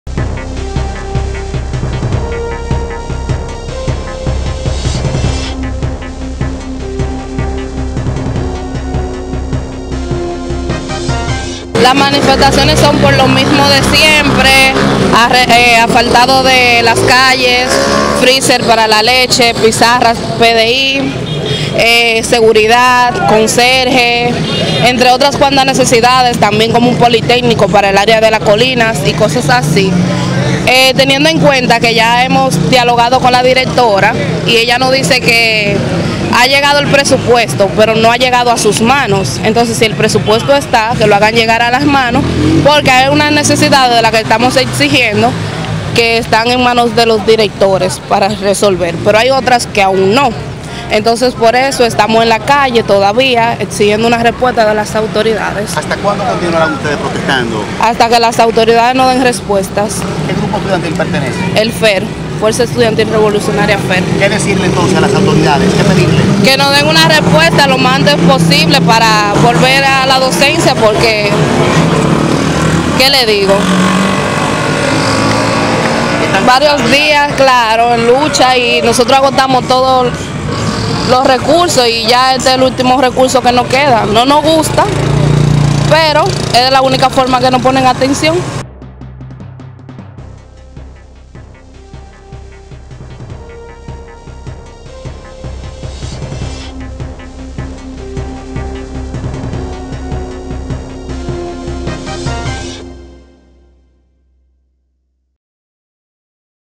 “Por lo mismo de siempre” vuelven a protestar en el Liceo Ercilia Pepín SFM
San Francisco de Macorís.- En demanda de diversas reivindicaciones, estudiantes del Liceo Ercilia Pepín volvieron a salir a protestar, paralizando el tránsito en la Avenida Libertad, en la mañana del miércoles 15 de febrero.

La protesta fue organizada por el Frente Estudiantil Revolucionario (FER), quienes indicaron que las demandas no han sido respondidas por las autoridades educativas, a pesar de los diálogos realizados y acuerdos arribados hace varios días.

Estefani Almonte, vocera del movimiento, dijo que las protestas obedecen a “lo mismo de siempre”; la necesidad de asfaltado, pizarras PDI, freezer para los alimentos líquidos que llegan al plantel, conserjes, personal de seguridad, entre otras necesidades que trasciende del centro, como la construcción de un politécnico para la zona norte.

La dirigente advirtió que estarán en las calles hasta tener las respuestas a sus demandas ya que han agotado procesos de diálogos y el único recurso que les queda es protestar en las calles debido a que es la única manera que los escuchan.